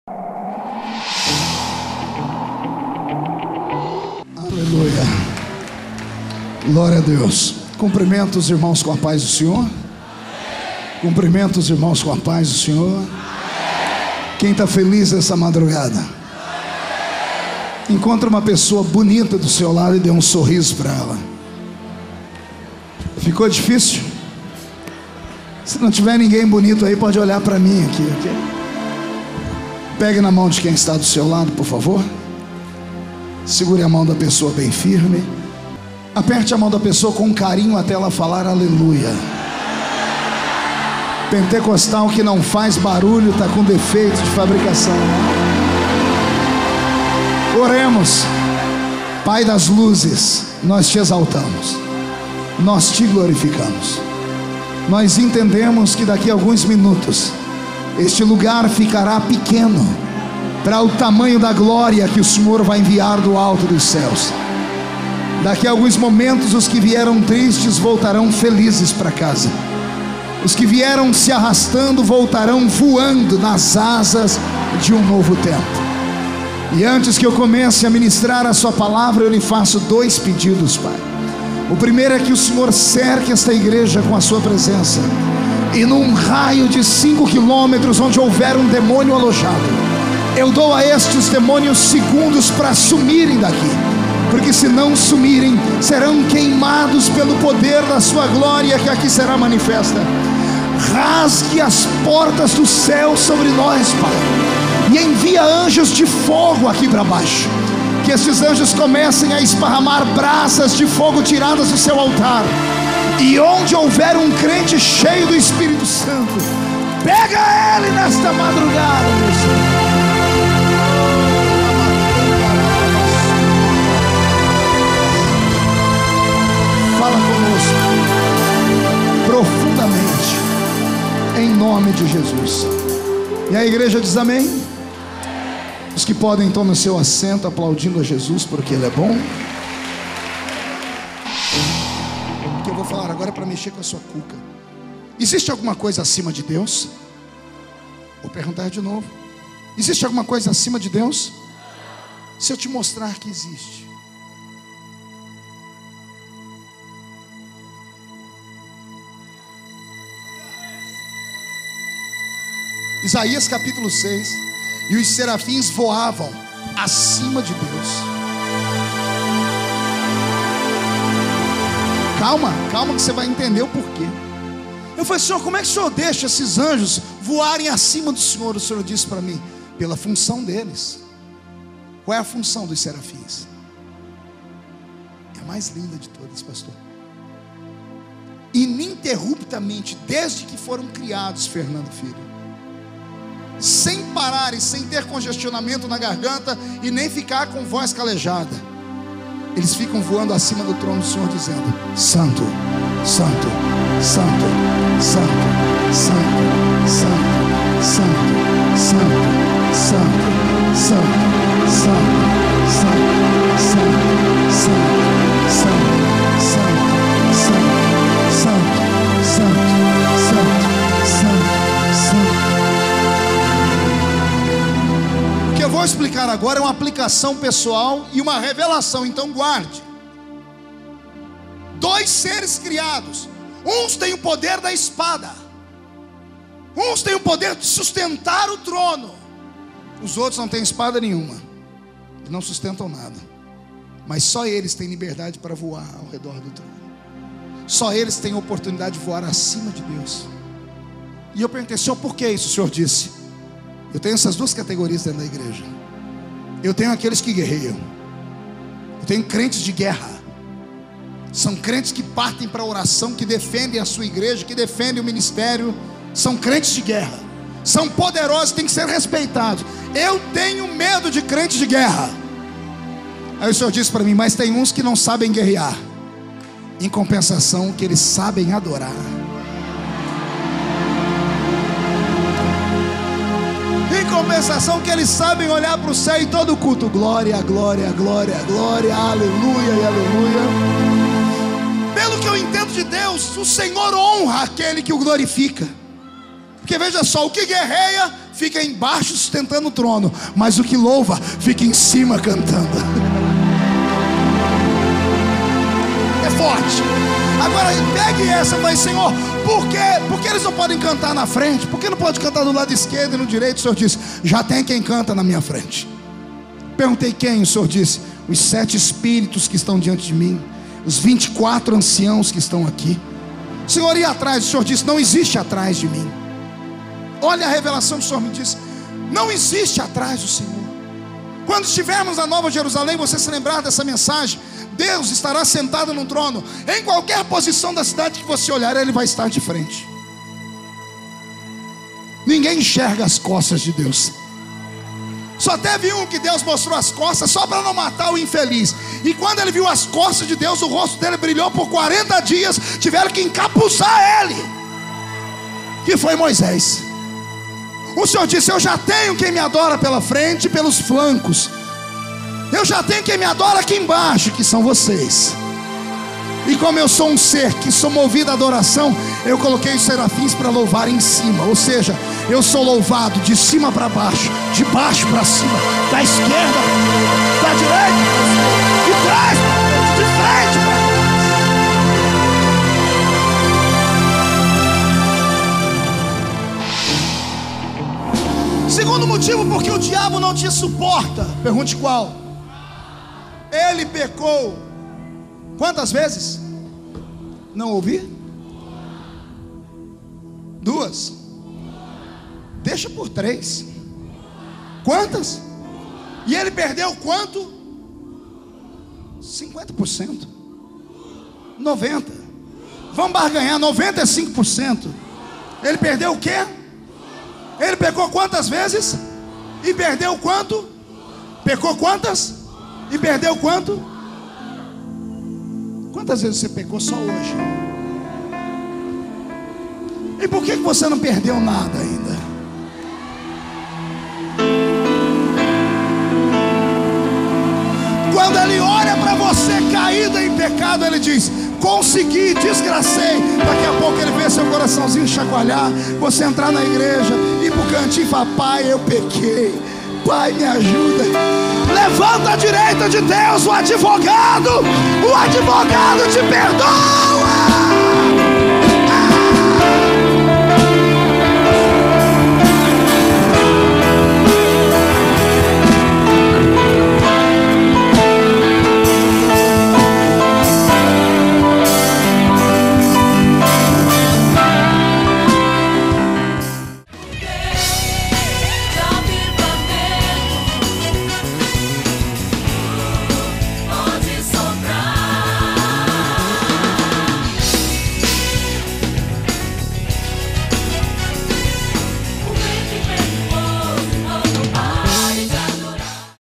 Pastor Marco Feliciano, O Poder da Adoração
A verdade sobre o Grande e Poderoso Querubim ungido da guarda vem neste DVD em forma de mensagem que beira a poesia. Como e por que  Lúcifer foi criado. Fez Deus o diabo? Por que o próprio Deus elogiou tanto essa criatura, o que eles tinham a mais que as outras? Onde ela estava quando arquitetou a rebelião celeste? Em que momento exato foi criado Miguel o arcanjo? Numa madrugada linda, na grande capital mineira, milhares de fieis se emocionaram com esta mensagem, agora é a sua vez! Assista!